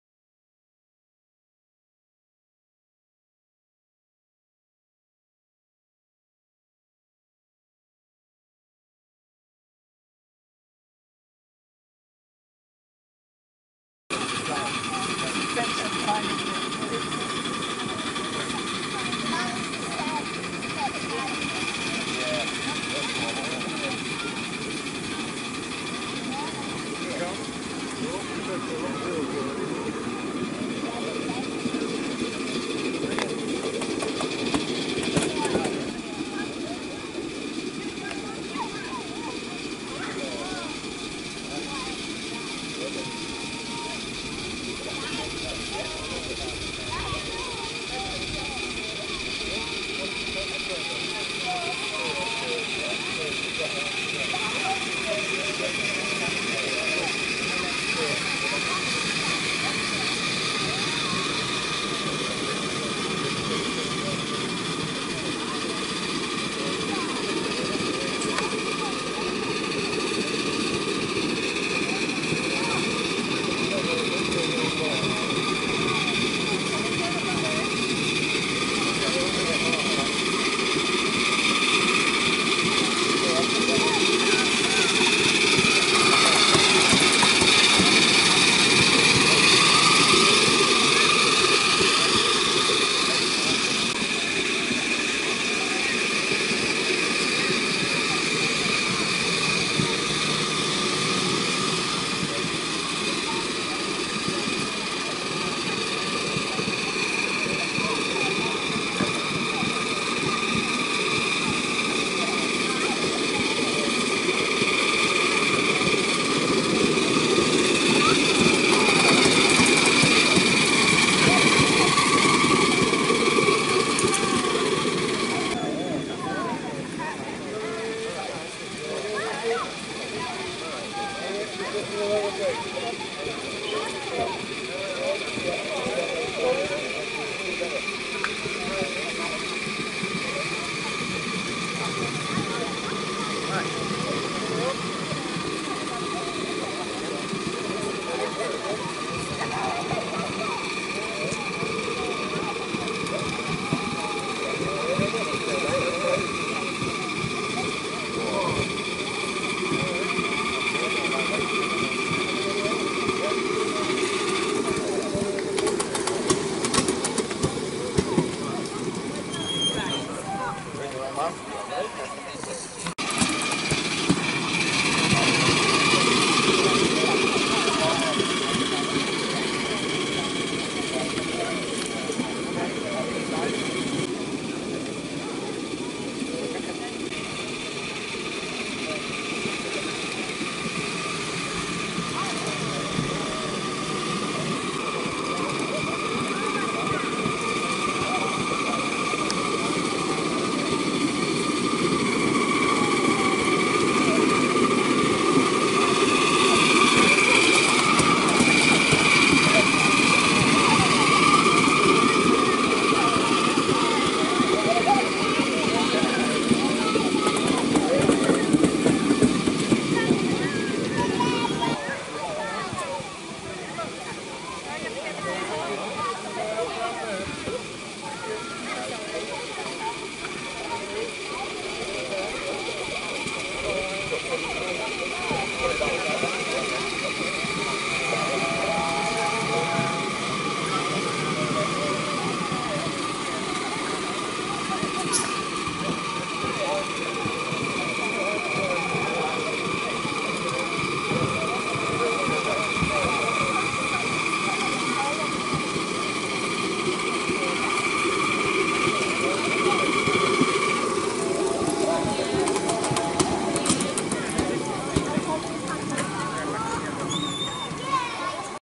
Crampton Locomotive
The Latest Video of the Crampton Locomotive in steam at Bradford Model Engineering Society's Miniature Railway. The Locomotive was built by Dr Jim Jennings and Painted by Mr Ken Shipley